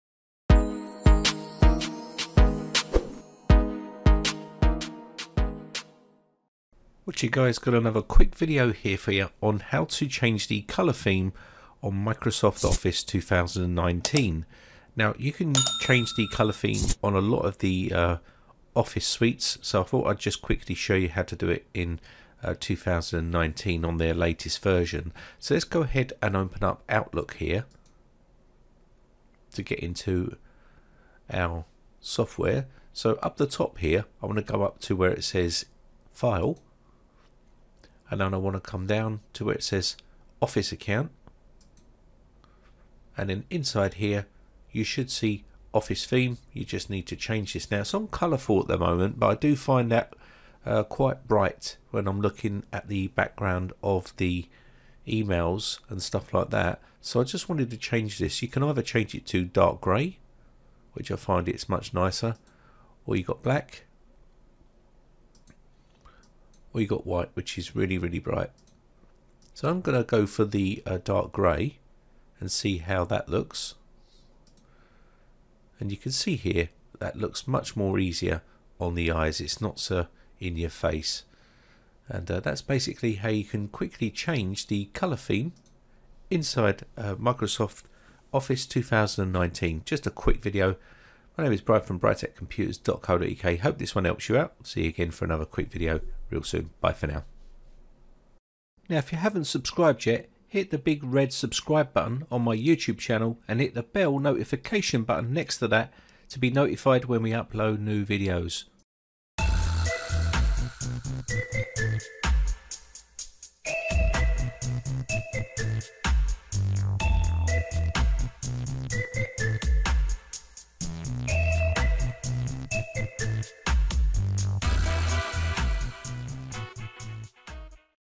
How to Enable Dark Mode in Microsoft Office 2019
Want to change the colour theme on Microsoft Office 2019? well I show you how, its real easy to do and protects your eyes. This also works in Office 365 in Windows and Mac.